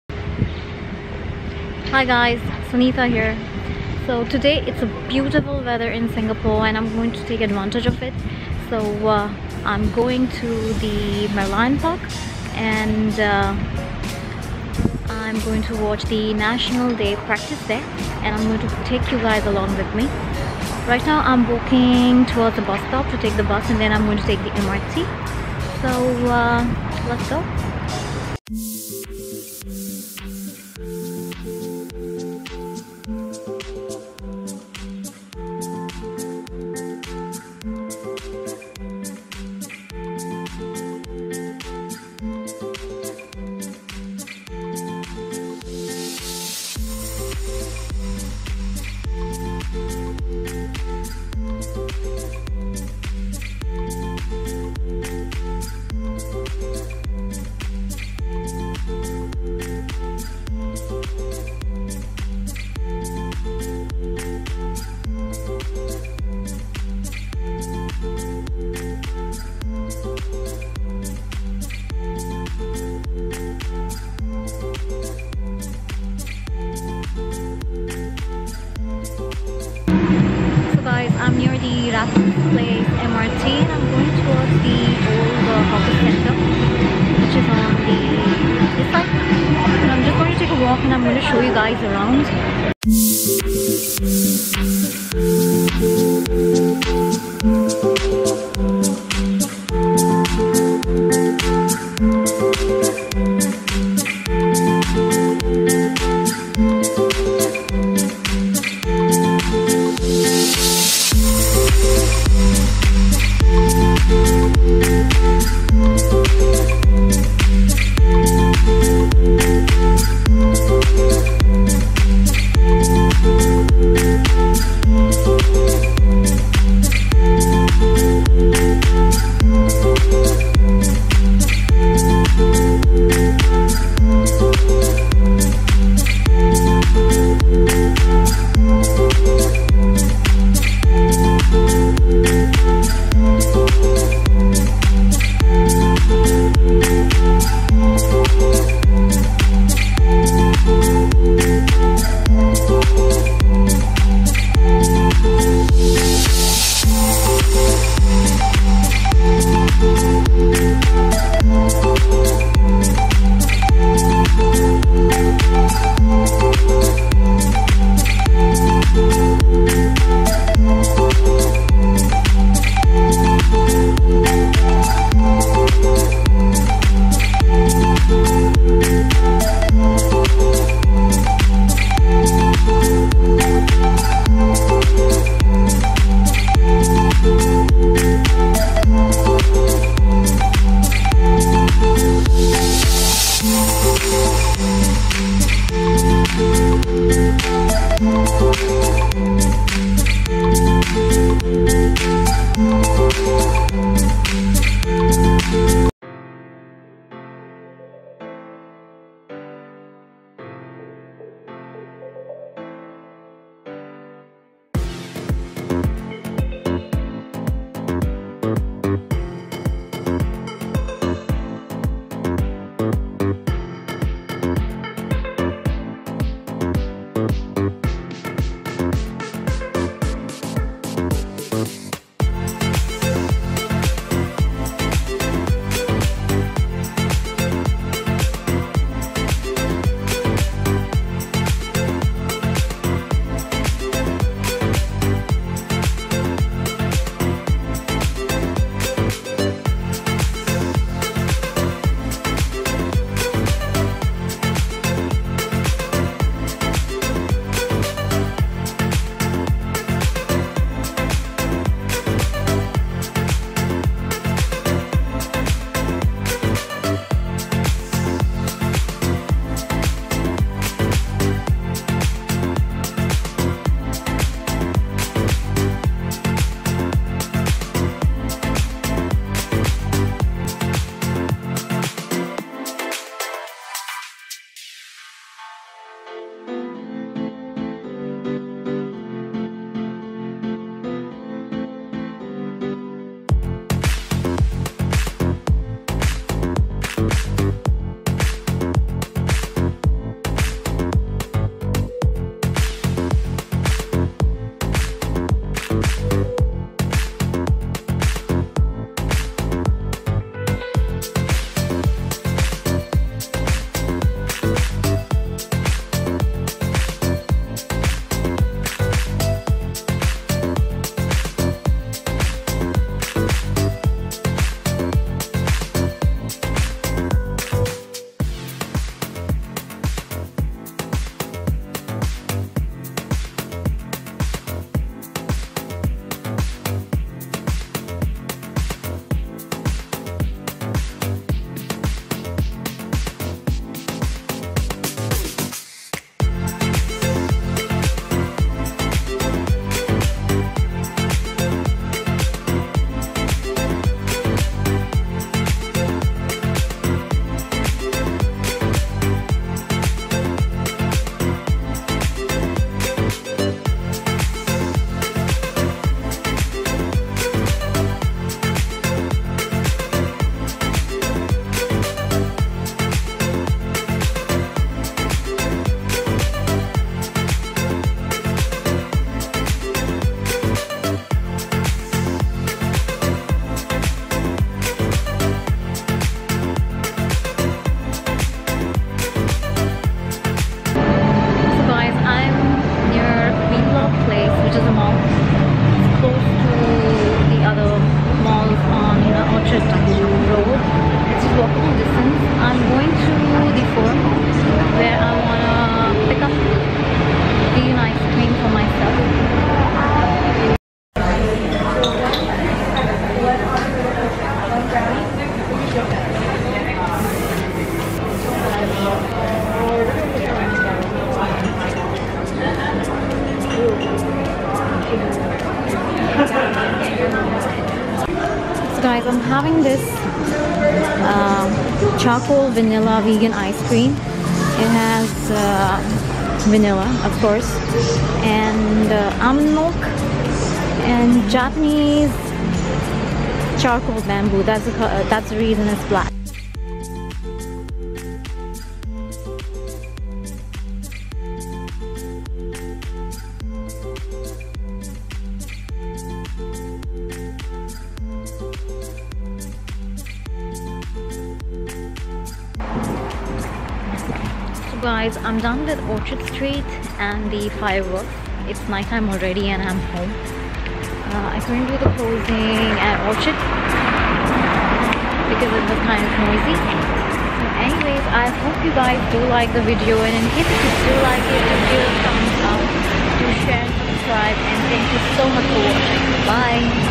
Singapore National Day Practice, Merlion park- Airshow# Fireworks#
Hi Everyone,

I am sharing with you all my experience of the National Day Practice. Lots of people who wont be able to attend the National Day Events come to see the practice which starts almost 1 mth before the National Day every Saturday at Merlion Park.

Also after the Merlion Park I enjoyed some vegan Icecream at Kind Kone B1- 27, The Forum Mall.

C u guys next time.